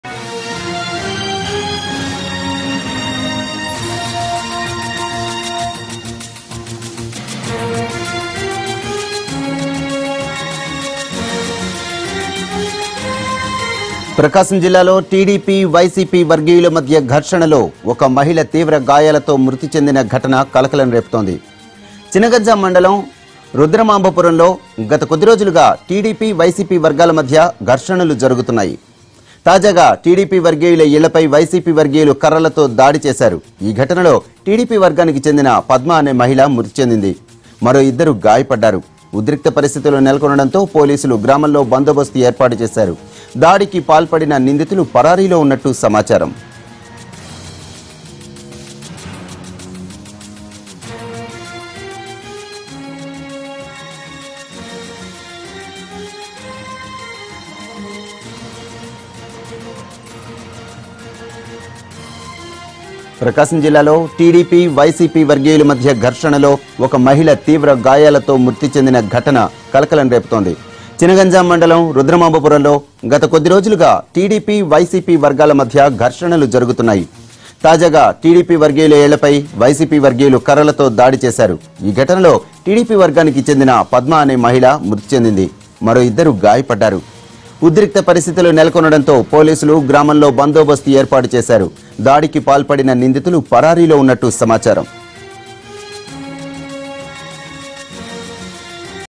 ప్రకాశం జిల్లాలో టీడీపీ, వైసీపీ మధ్య ఘర్షణ.. గాయాలతో టీడీపీ కార్యకర్త మృతి || NTV
ప్రకాశం జిల్లాలో టీడీపీ, వైసీపీ మధ్య ఘర్షణ.. గాయాలతో టీడీపీ కార్యకర్త మృతి